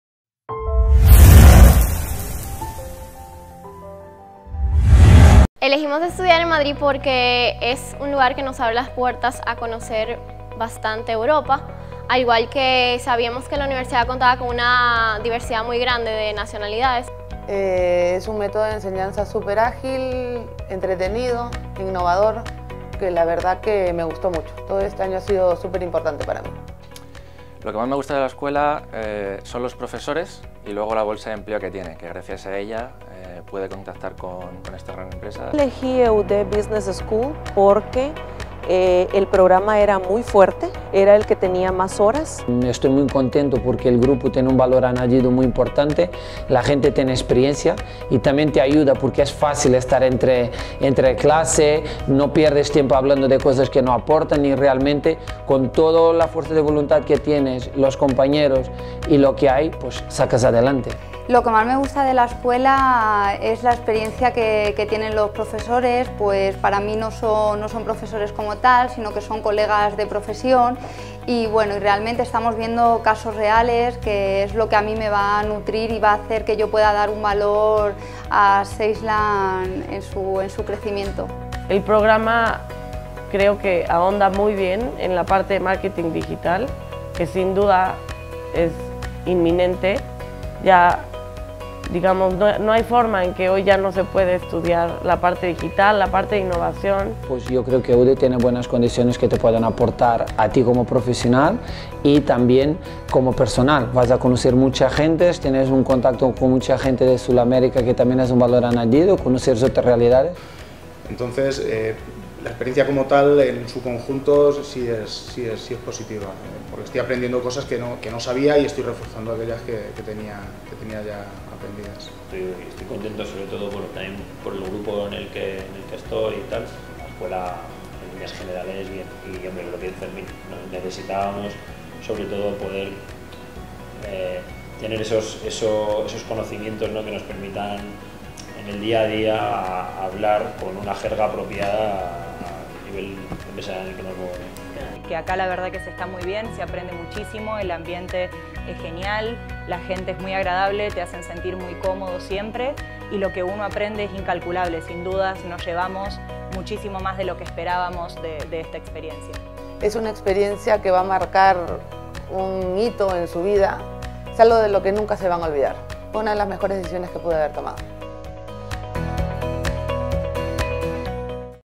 Testimoniales EUDE Business School
EUDE Business School lleva más de 20 años en el ámbito de la educación de postgrado. Durante todo ese tiempo ha recibido alumnos de múltiples nacionalidades. Personas recién licenciadas, directivos de grandes compañías e incluso emprendedores.

Descubre a través de los siguientes estudiantes cuáles fueron las razones que les motivaron a estudiar aquí, por qué es importante la formación de postgrado y cuáles son sus planes de futuro tras finalizar sus estudios.